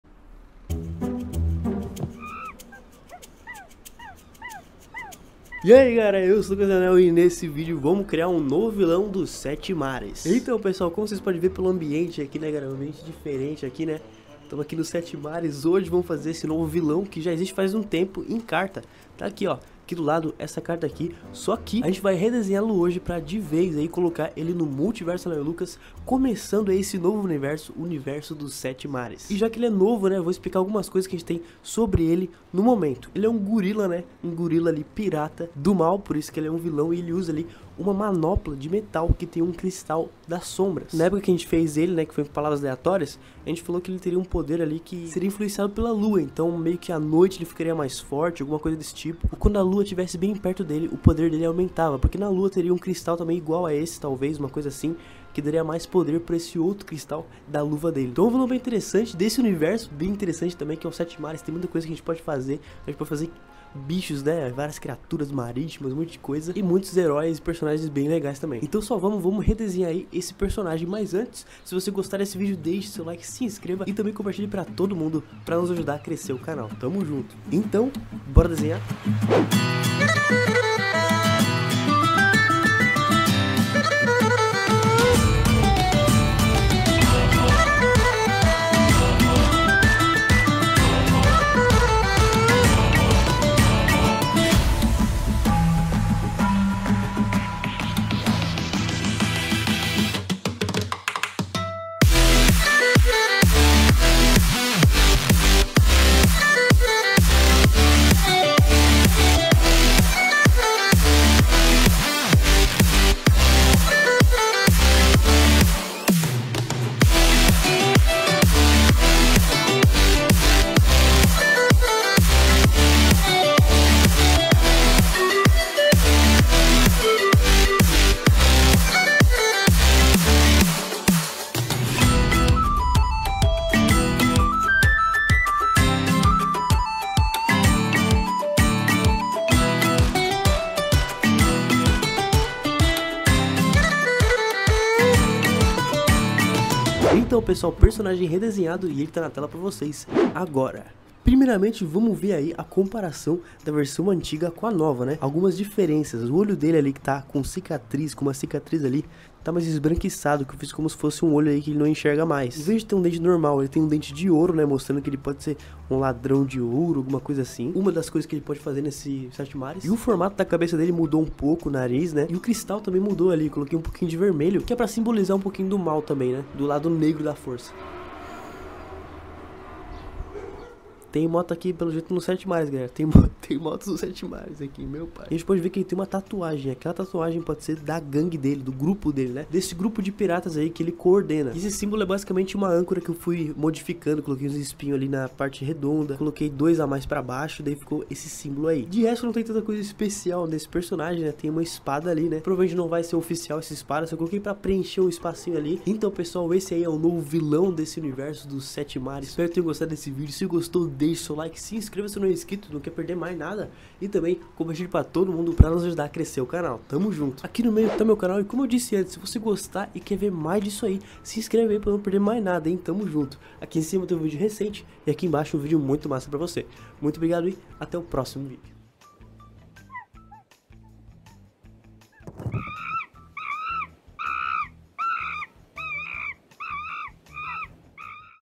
DESENHANDO UM NOVO VILÃO PIRATA!!! | Darknape | Lionel Lucas
Neste vídeo eu redesenho o esquecido vilão dos 7 mares, Darknape!
Seja membro do canal e ganhe benefícios ELETRIZANTES!

Partes do Vídeo:
00:00 - Início
01:18 - Desenho
02:26- Análise
03:25 - Final